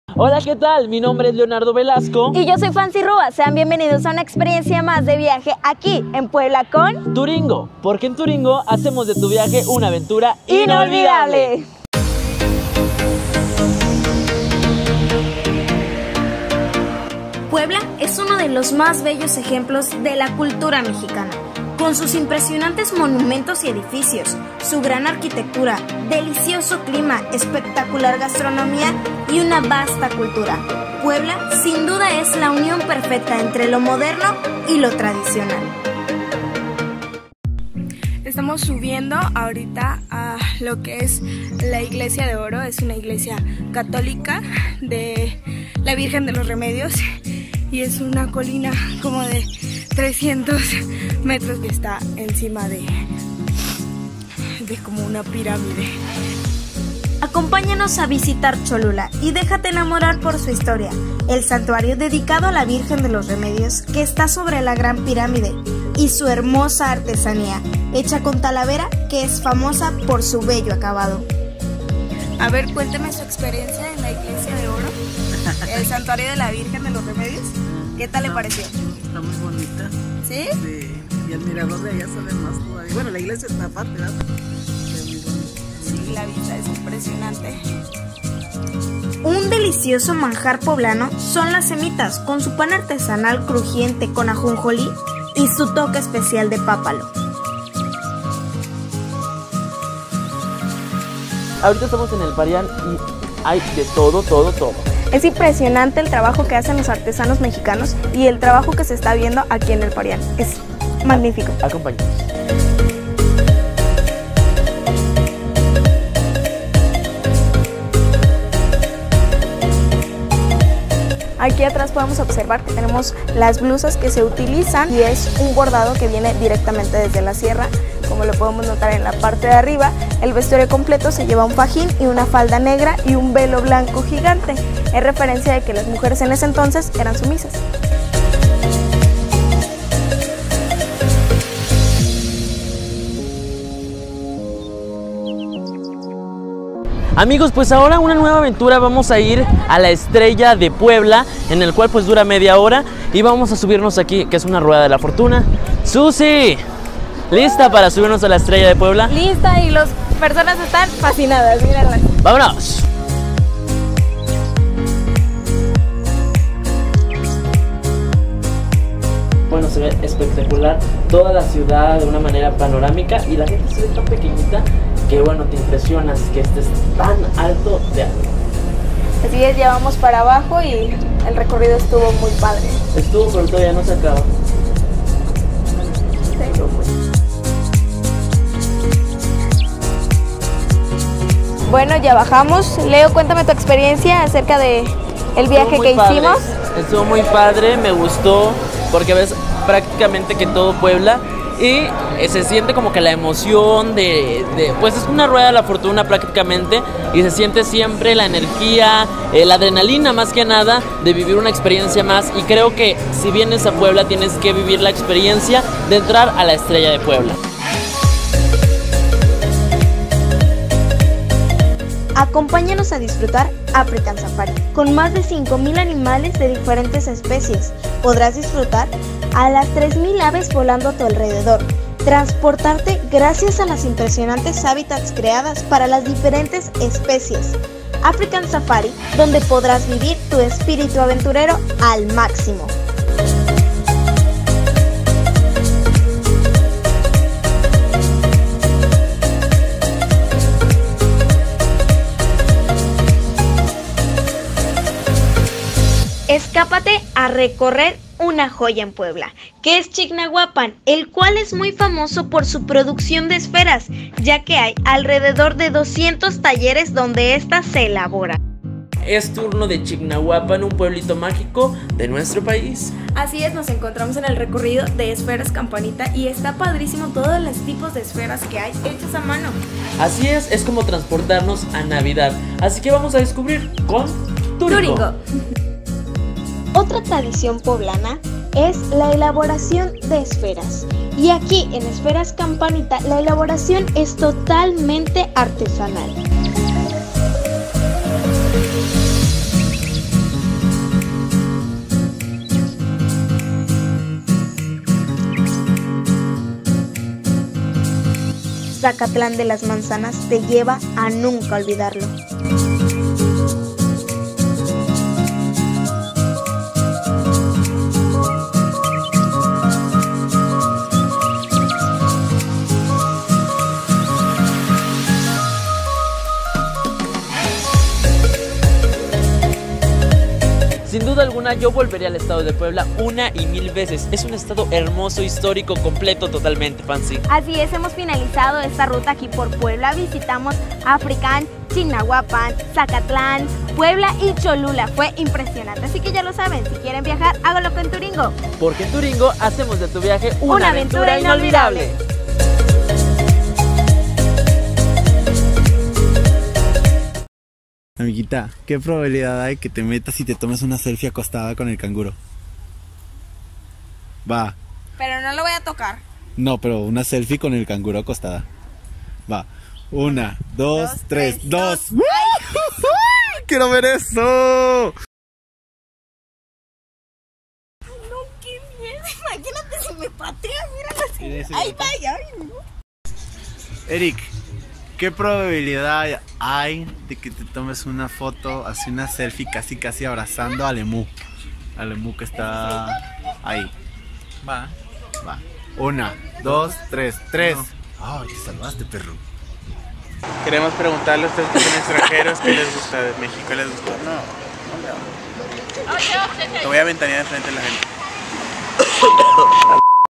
puebla experienciadeviaje turyngo
Visita con nosotros Puebla, zacatlan, chignahuapam, africam safari y cholula